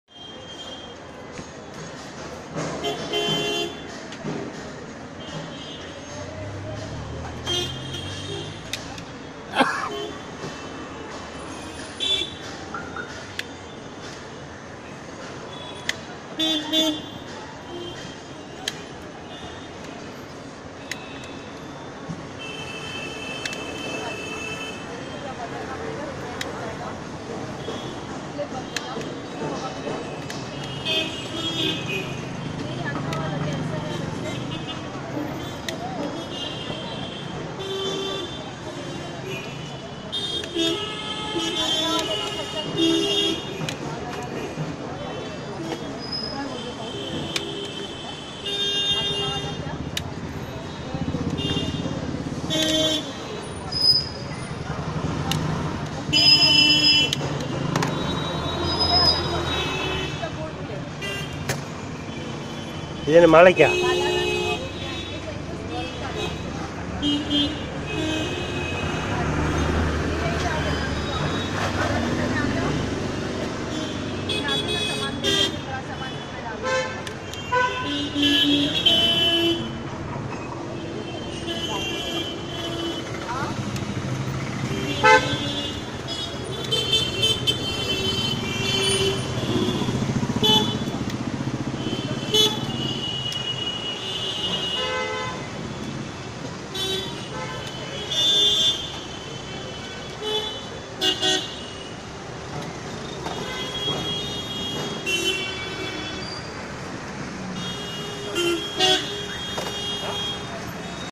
Garland making art in Mumbai - 8 | மும்பையில் பூக்கட்டும் கலை - 8
A different type of flower stringing and garland making is happening in Mumbai. Watch how they are doing this.

Video captured by Annakannan.

மும்பையில் புதிய வகையில் பூக்கட்டுகிறார்கள். அது எப்படி என்பதை இந்தப் பதிவில் பாருங்கள்.

காணொலிப் பதிவு: அண்ணாகண்ணன்